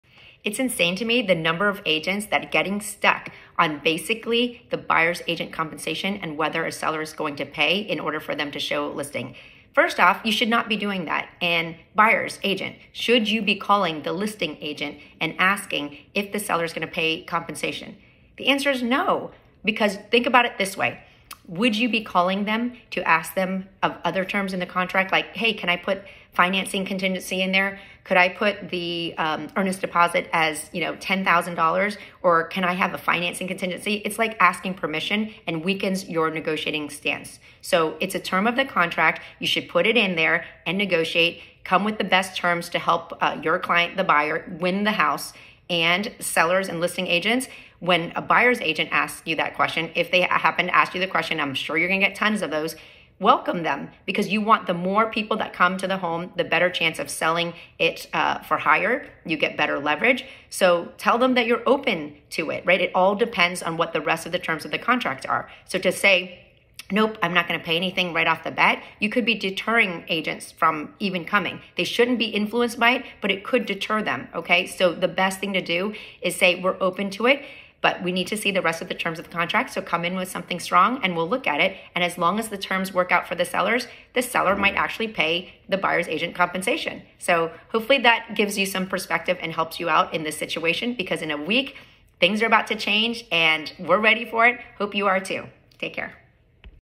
NAR Settlement & Agent Commission Tips for Buyer’s Agents & Listing Agents
My Realtor friends...are you ready for August 17th? 🤔

As a Buyer’s Agent, should you be calling the listing agent to ask if the seller is going to pay compensation? No...that could compromise your position & weakens your negotiation power.

As a listing agent, should you inform buyer’s agents that the seller isn’t offering compensation? No...that could cause friction & may limit the possibilities for sellers.

Compensation has always been negotiable—it’s just now explicitly stated in the contract just like all the other terms, which enhances transparency. Let’s not make this more complicated than it needs to be. Yes, it’s different from what we’re used to, but that doesn’t mean it’s a bad thing. Stay confident, be a strong negotiator & secure the best terms for your clients, whether you’re representing the buyer or the seller. You got this!!! 🤗🏡